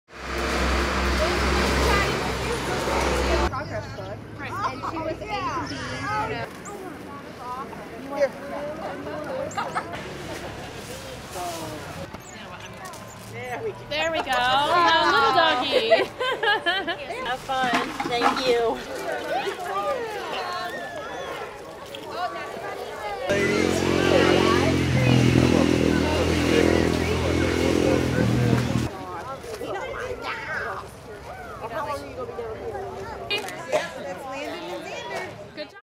New Concord Halloween
The Village of New Concord Celebrates Halloween with a costume contest and trick or treat on main street.